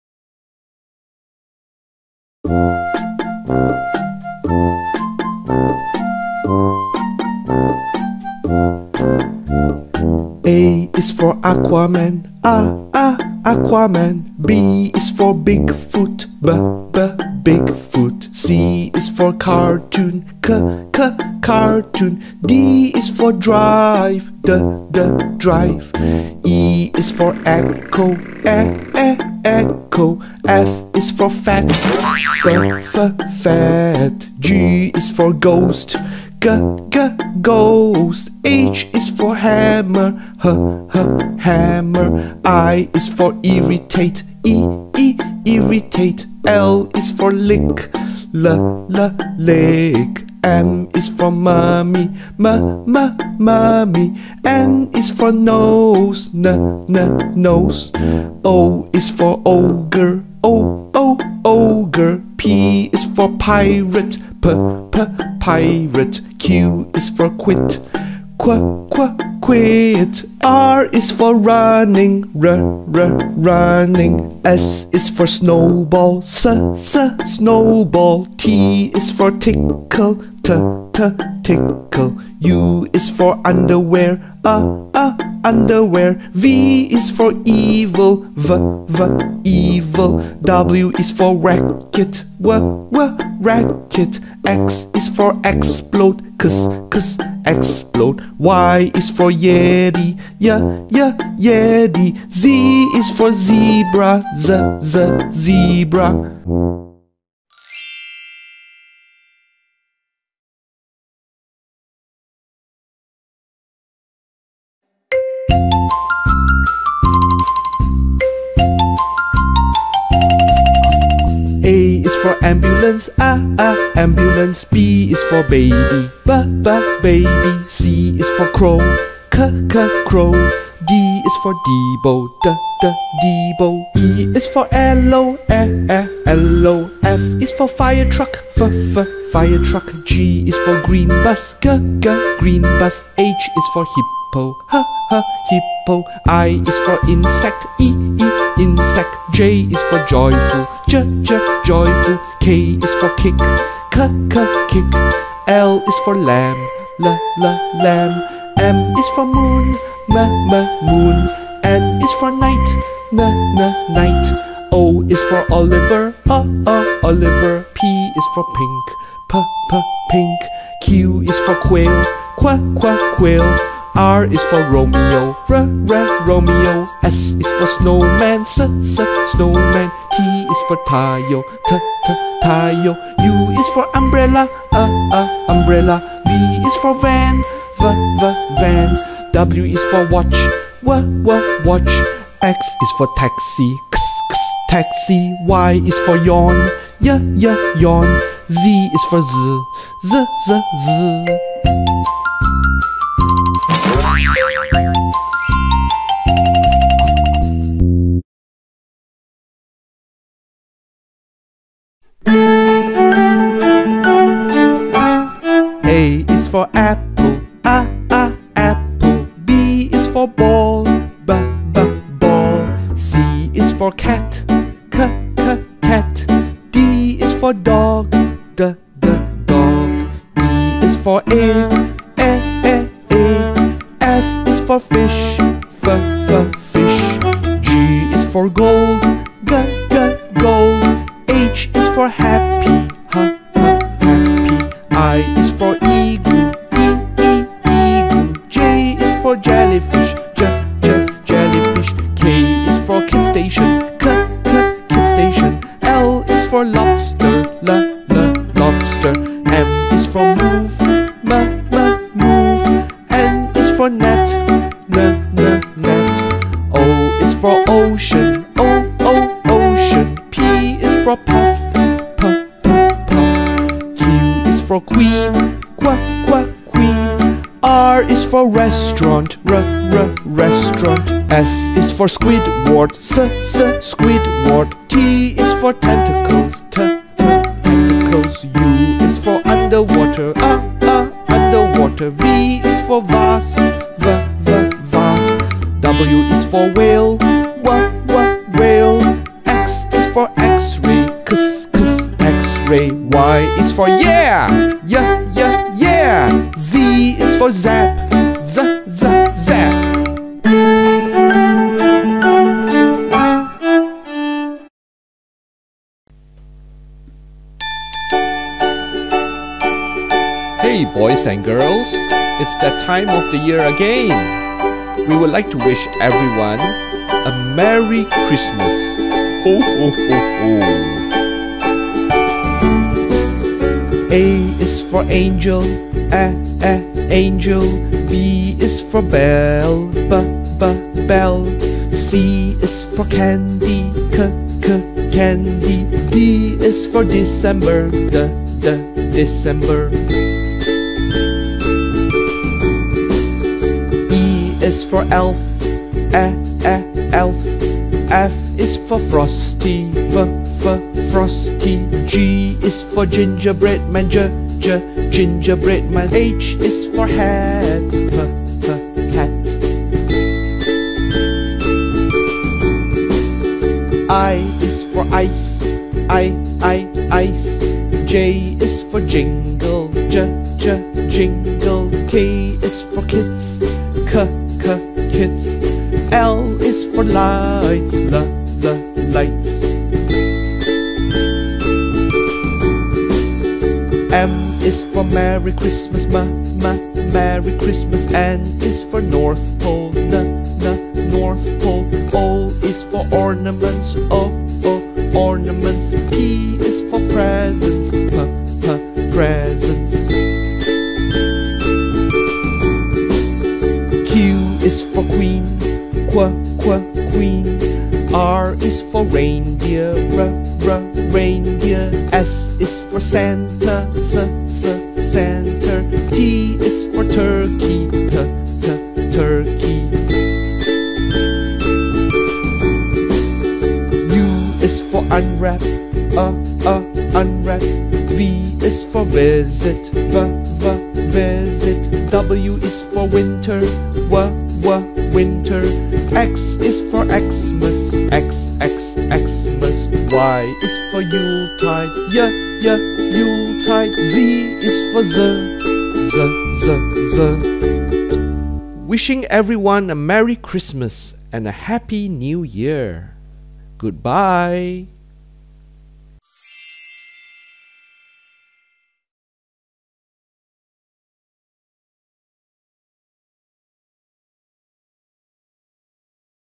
Phonics Song | kidzstation Compilation A to Z Cartoons
This is a collection of phonics songs with a picture for each letter.  This is designed to help children learn the sounds of the letters in the English alphabet.